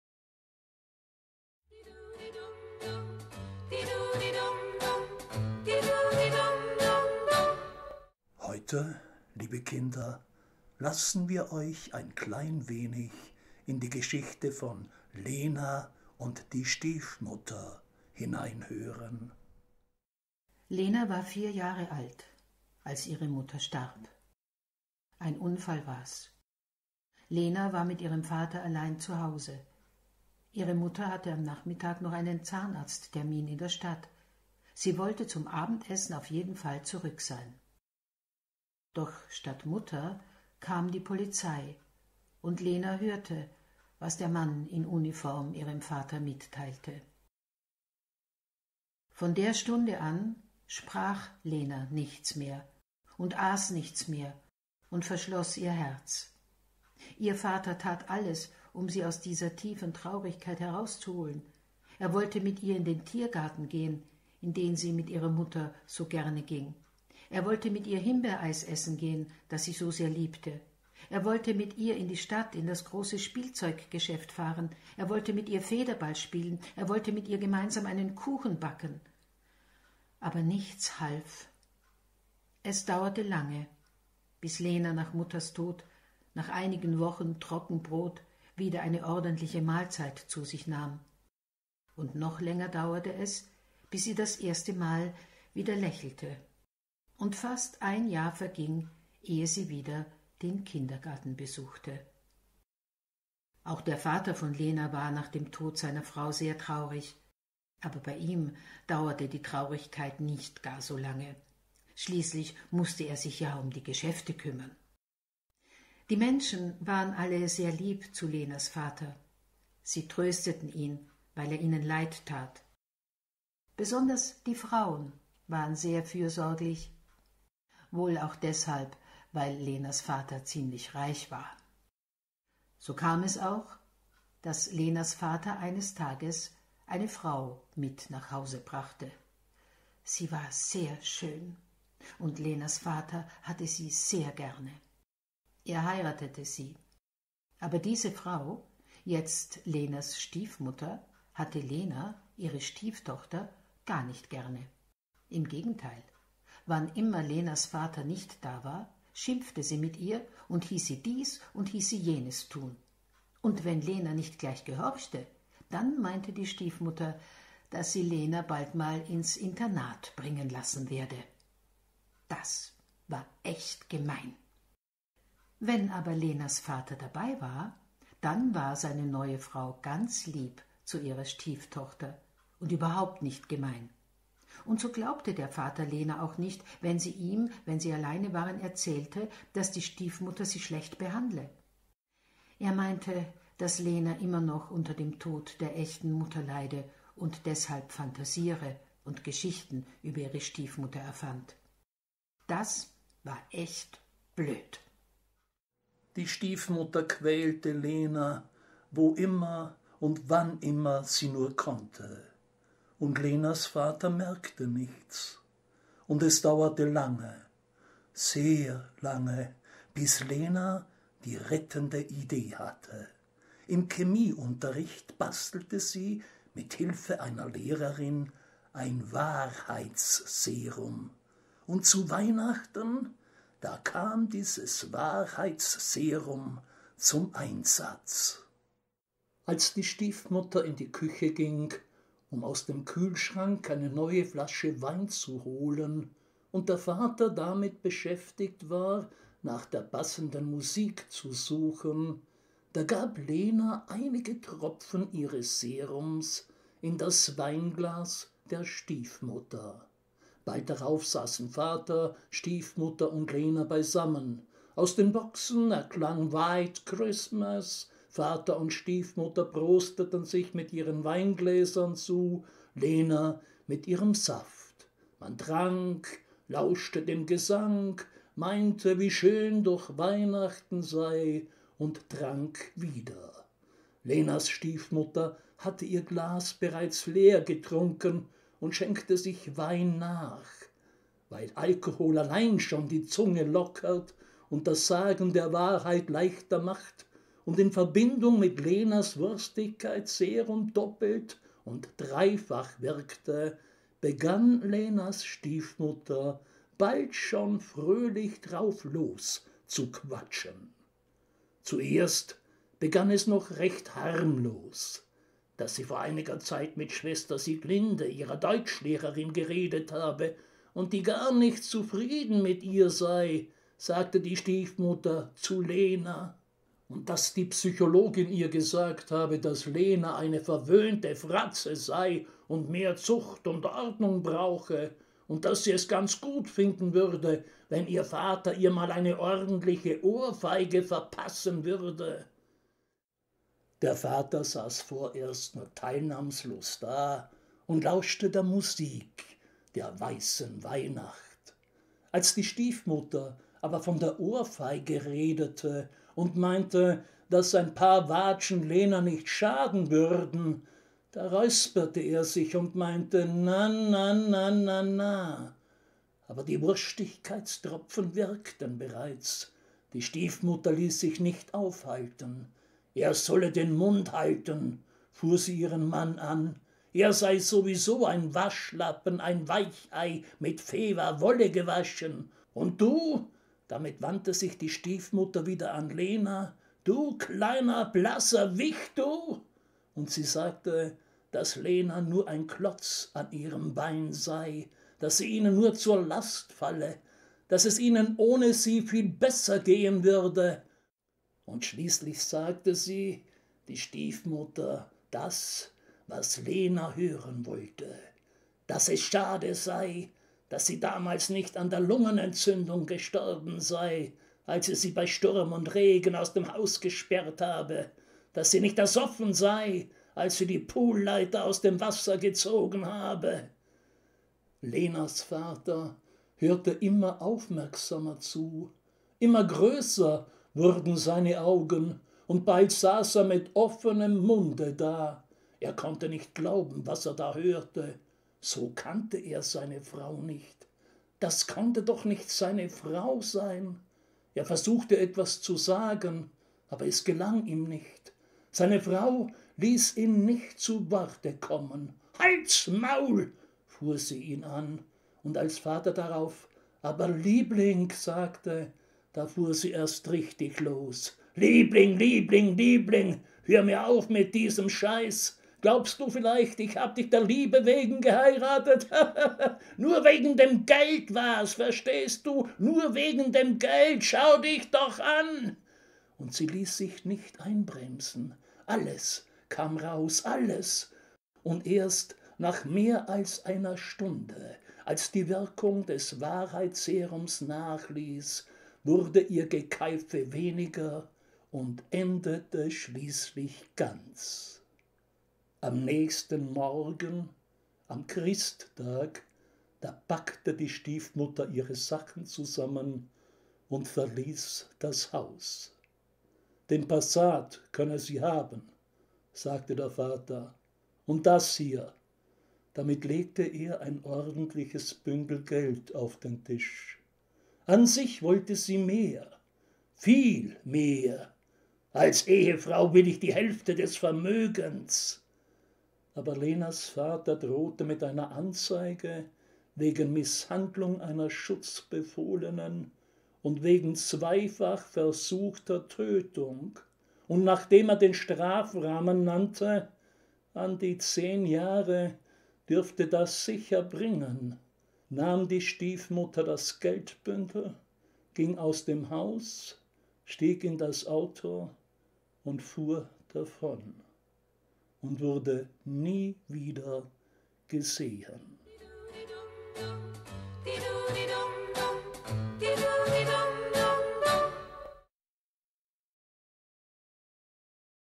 GESCHICHTE 6: Lena und die Stiefmutter
Im März 2020 (während des Lockdowns) begann der alte Opa mit seinen Enkelkindern via Internet Geschichten zu schreiben. Daraus ist das Buch "Märchen aus Corona-Tagen" entstanden. Auf dieser Seite könnt ihr in einzelne dieser Geschichten hineinhören. Heute in die Geschichte von Lena und ihrer Stiefmutter.